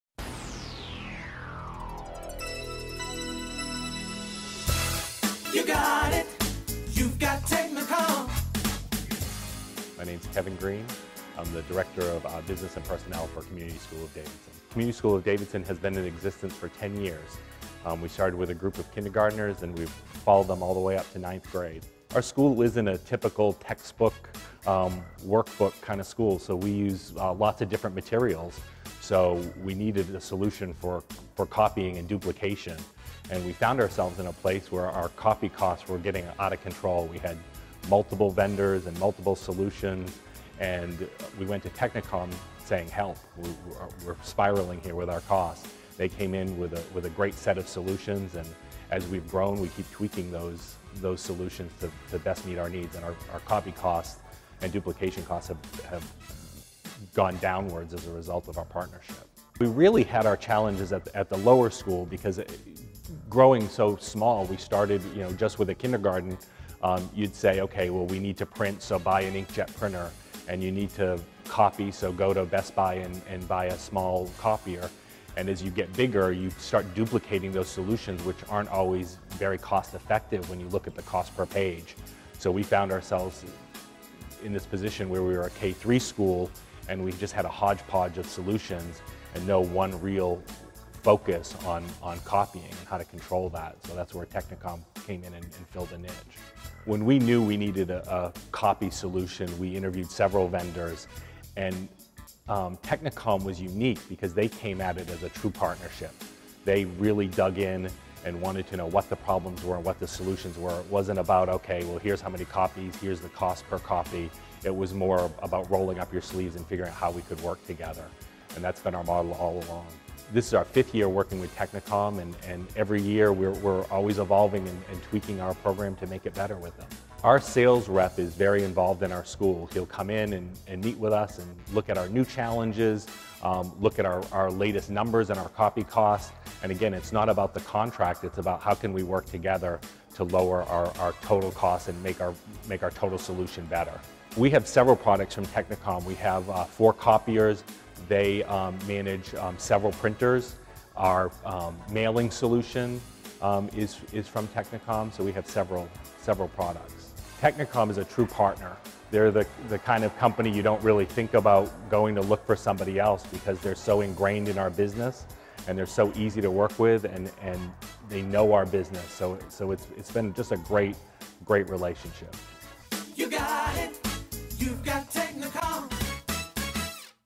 Community School of Davidson
Opened in 2001 at Lake Norman Baptist Church, Community School of Davidson has grown to become a North Carolina Charter School, providing education to children in Kindergarten up to 9th grade. Community School of Davidson believes "that every student can and will succeed in ways that reflect his or her own aptitudes and interests." The school uses the principles of The Basic School, a comprehensive plan that strives to incorporate research proven practices in education.